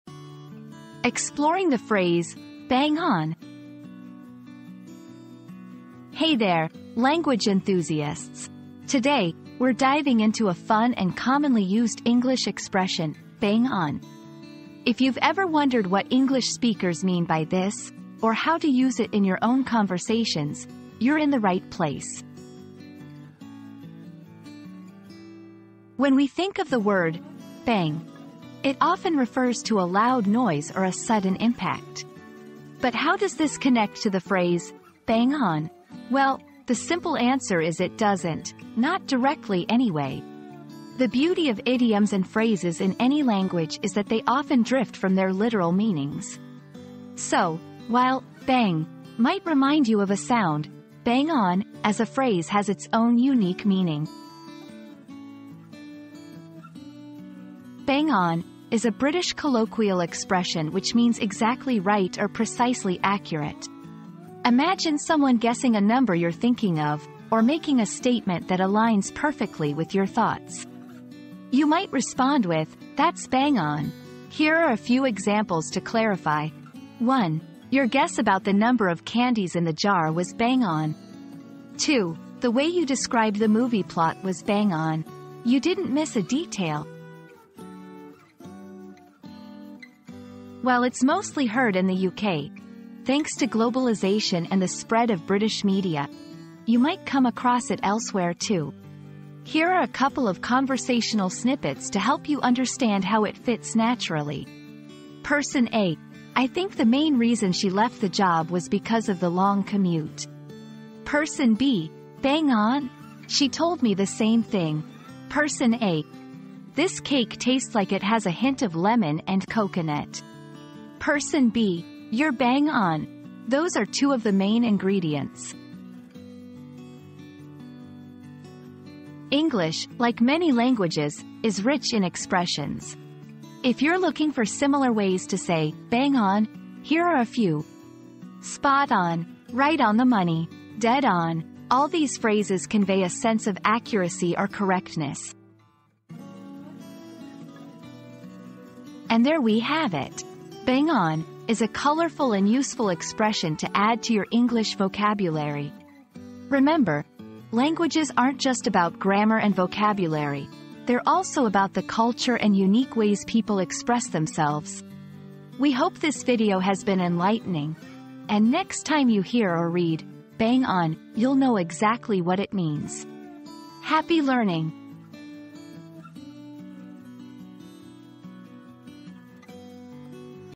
Exploring the Phrase "Bang On!"
Unveiling the Power of 'Bang On!' • Discover the true meaning and impact of the popular phrase 'Bang On!' in this intriguing exploration. From its origins to its modern usage, this video sheds light on the significance and versatility of this catchy expression. Join us on this linguistic journey and unlock the potential of 'Bang On!'

00:00 • Introduction - Exploring the Phrase "Bang On!"
00:25 • The Literal Meaning
00:57 • The Idiomatic Meaning
01:30 • Usage in Conversations
02:08 • Similar Phrases